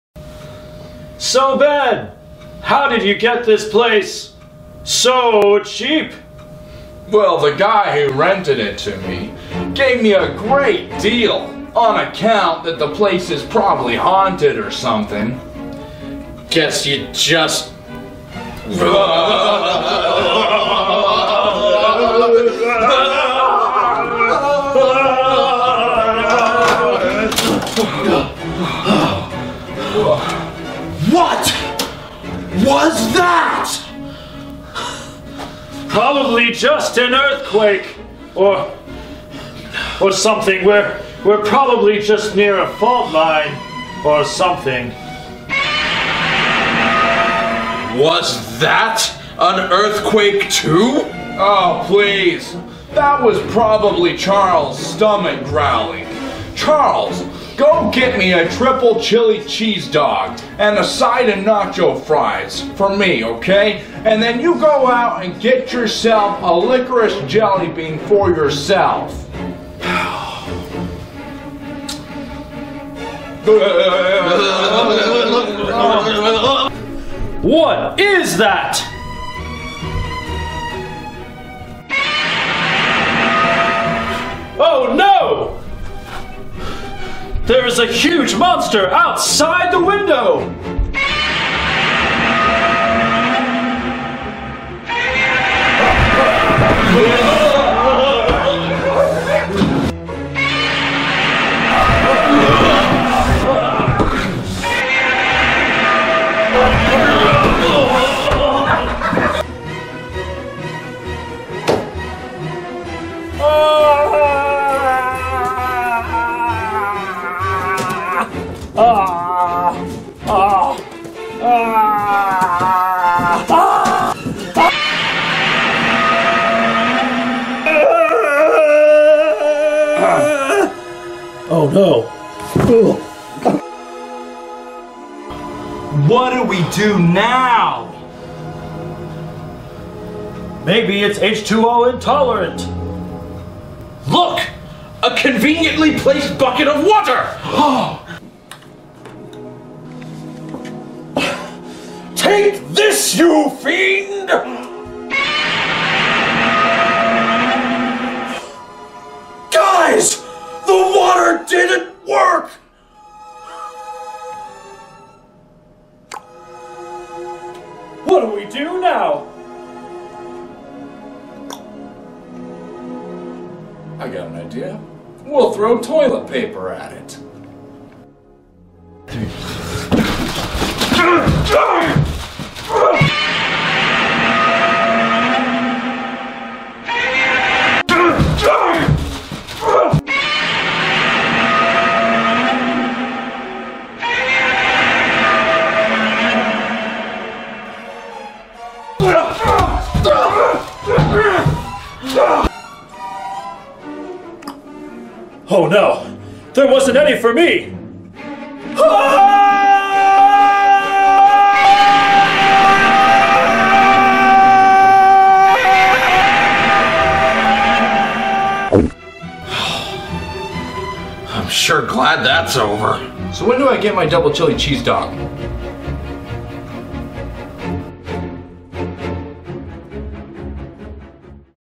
Classic Monster Movie
A cheesy and funny take on old fashioned monster movies.